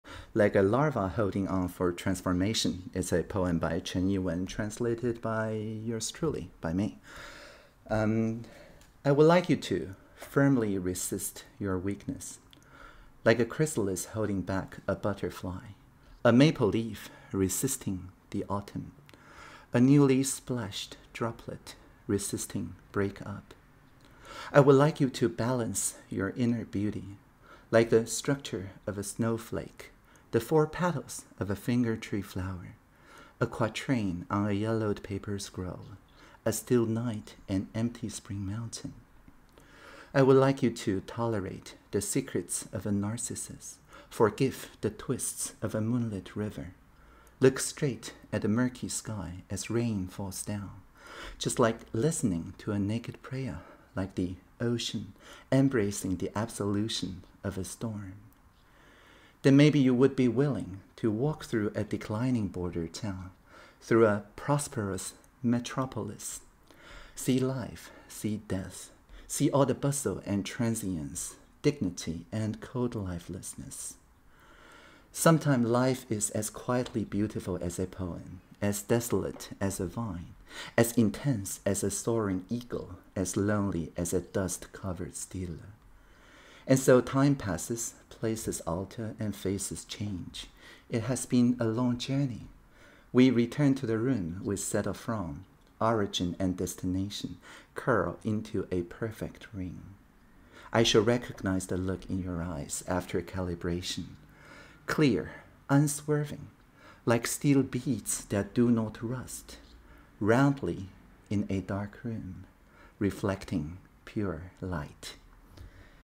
Like a larva holding on for transformation / Reading: Audrey Tang・Art: Saya Mitake
This is a poem recited by Audrey Tang (Minister for Digital, Taiwan Government.). I received the audio data (CC0) from her. It was so beautiful that I'm sharing it here. The painting that accompanied the poem is of the famous Japanese cherry tree, Someiyoshino.

Like a larva holding on for transformation / Chen, Yi-Wen

I would like you to
firmly resist your weakness
like a chrysalis holding back a butterfly, a maple leaf resisting the autumn
a newly splashed
droplet resisting breakup
balance your inner beauty
like the structure of a snowflake, the four petals of a finger tree flower
a quatrain on a yellowed paper scroll:
“a still night, an empty spring mountain”
tolerate the secrets of a narcissus
forgive the twists of a moonlit river
look straight at the murky sky as rain falls down
just like listening to a naked prayer
like the ocean
embracing the absolution of a storm
Then, maybe you would be willing
to walk through a declining border town
through a prosperous metropolis
see life, see death
see all the bustle and transience
dignity and cold lifelessness
Sometimes, life is
as quietly beautiful as a poem, as desolate as a vine
as intense as a soaring eagle
as lonely as a dust-covered stele
And so time passes, places alter, faces change
it has been a long journey
we return to the room we set off from
origin and destination curl into a perfect ring
I shall recognize the look in your eyes after calibration:
clear, unswerving
like steel beads that do not rust
roundly, in a dark room
Reflecting pure light